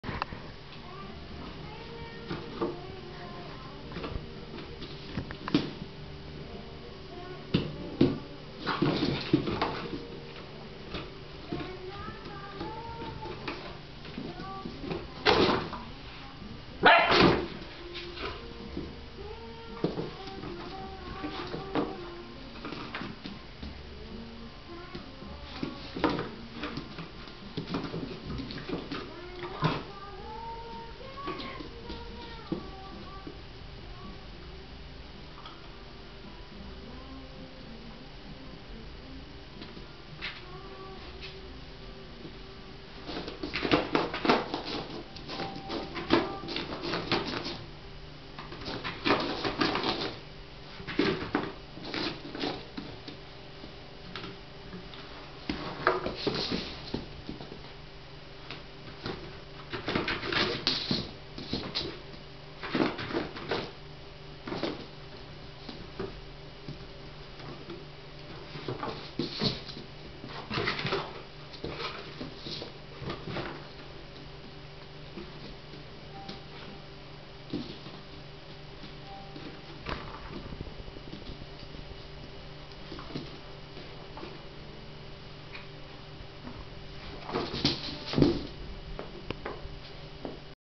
Hidde met Doos
Onze drenttsche patrijs pup van bijna 9 weken aan het spelen met een kartonnen doos.
Aan zijn staar zie je dat hij het ook wel spannend vind.